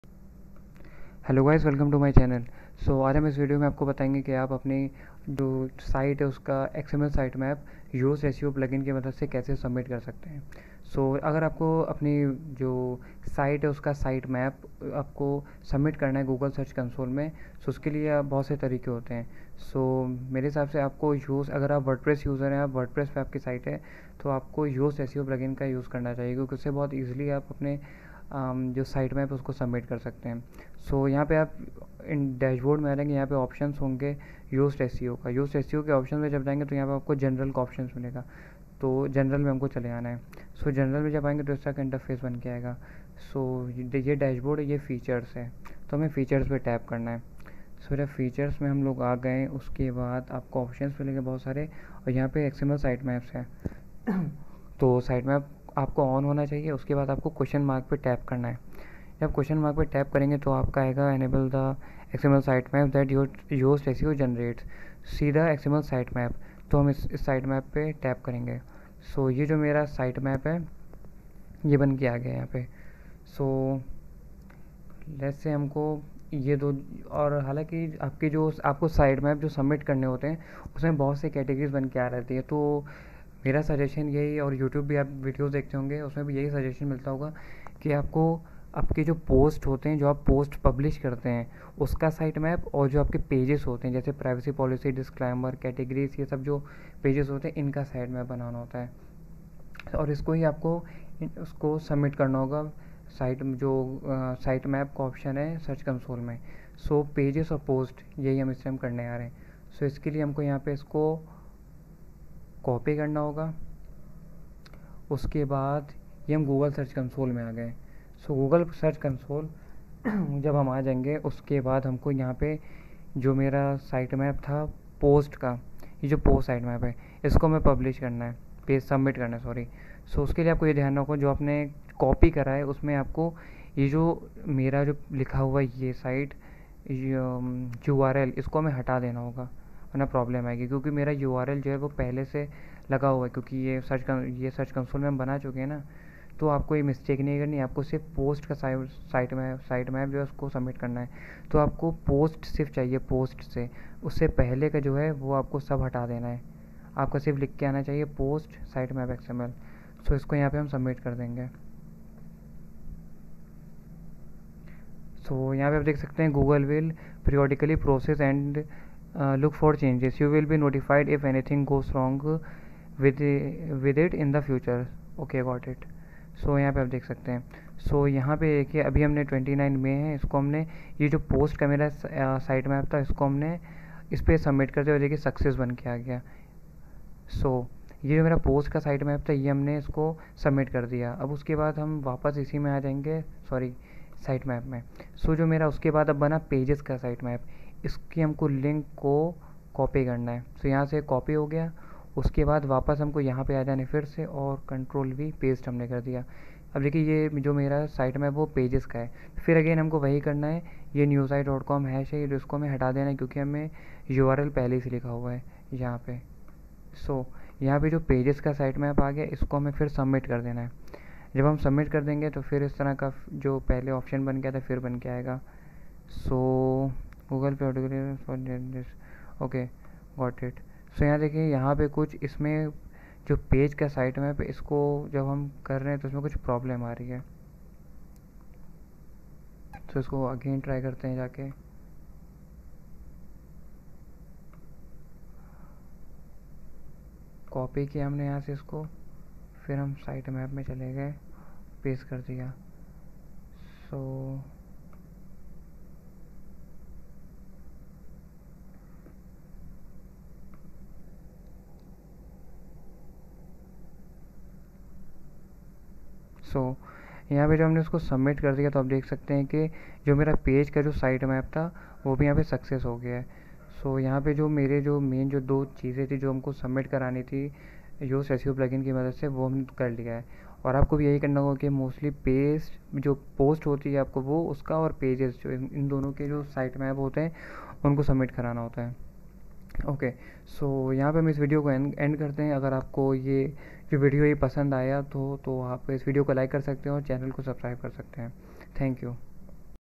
How to get sitemap from Yoast seo plugin and submit to google search console | Murtuza Husain
In this video I'm going to tell you that how you can get sitemap from yoast seo plugin and then how you have to submit it on Google Search Console.